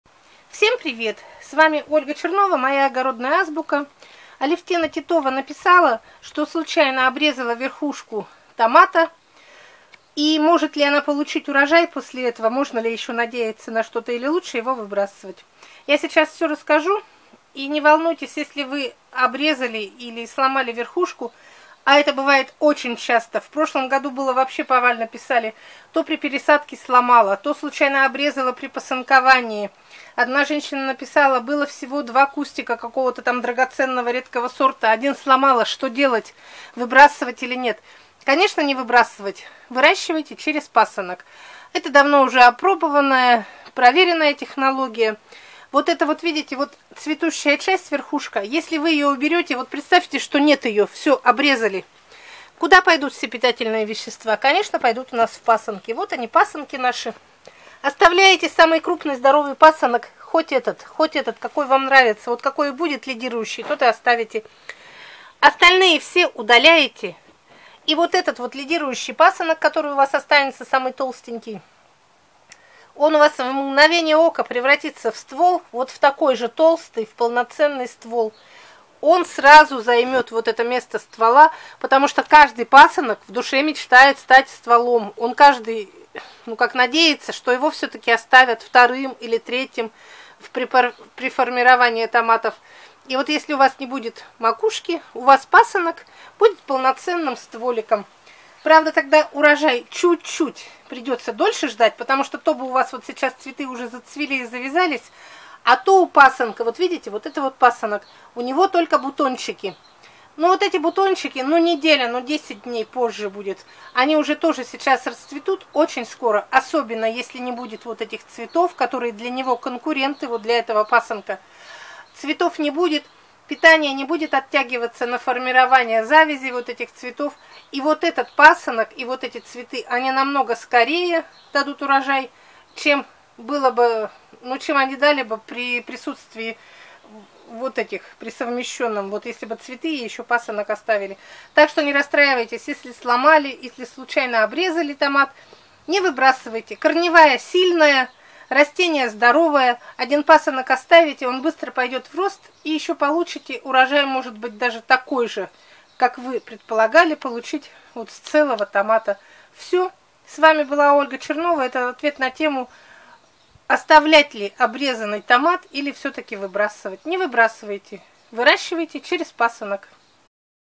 СЛОМАНЫЙ ИЛИ ОБРЕЗАННЫЙ ТОМАТ МОЖНО ВЫРАЩИВАТЬ ЧЕРЕЗ ПАСЫНОК. Ольга Чернова.
Бывают ситуации, когда случайно сломанный томат очень ценен для нас. Сохранить куст и получить урожай можно,вырастив этот томат через пасынок.